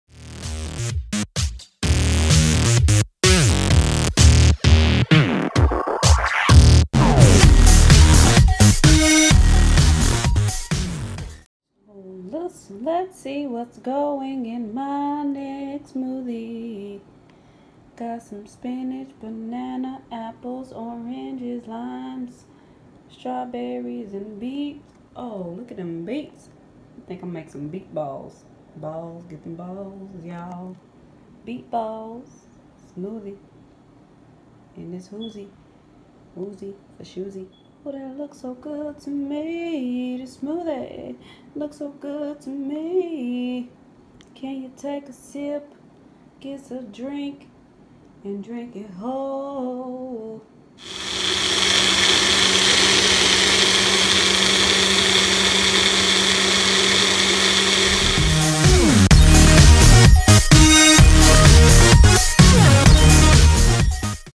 How to: IRON PACKED VEGAN SMOOTHIE
Low on Iron but want to know how to make an easy smoothie for anemia or Iron Deficiency?

Recipe calls for:
1 1/2 c Spinach but preferably Dandelion greens
1 small beet
1/4 c Strawberries
1 medium banana
1 orange
1 lime or 2 key limes
2 apples-juiced

DIRECTIONS:

Juice 2 apples in blender.
Blend all other ingredients until smooth or desired consistency